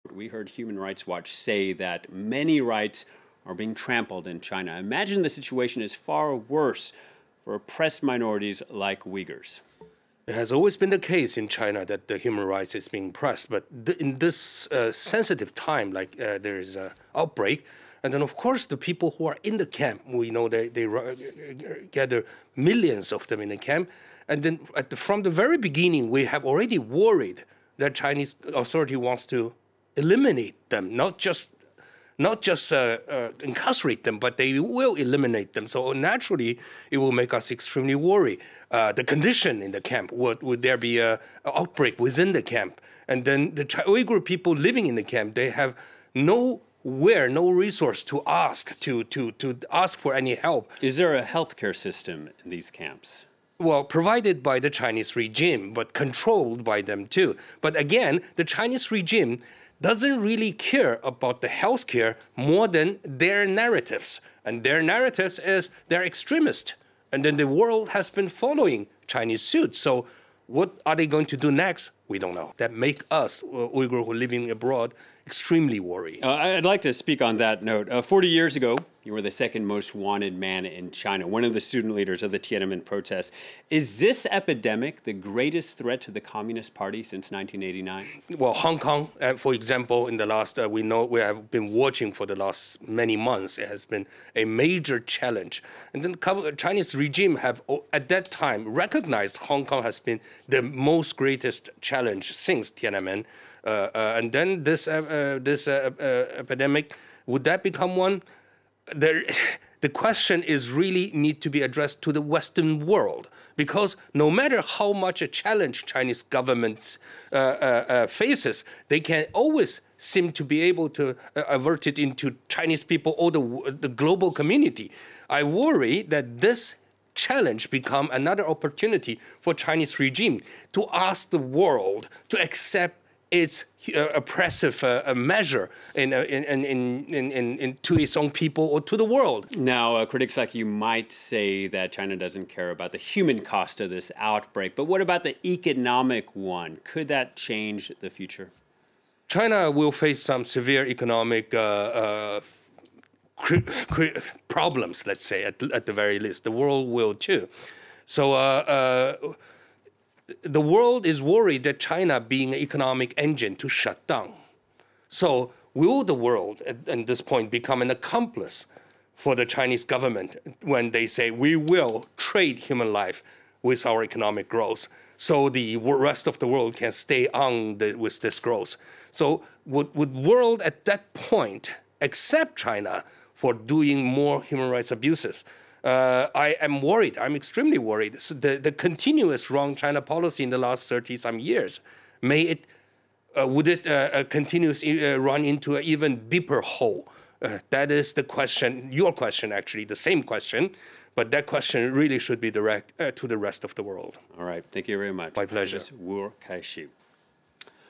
Coronavirus outbreak: Uighur activist Wu'er Kaixi says epidemic worsens minority's plight
What are the effects on millions of ordinary citizens, as well as oppressed minorities like the Uighurs? We also ask about the political costs for the Chinese leadership as the country's handling of the COVID-19 crisis comes under scrutiny. To discuss these issues, we're joined by Uighur activist Wu'er Kaixi.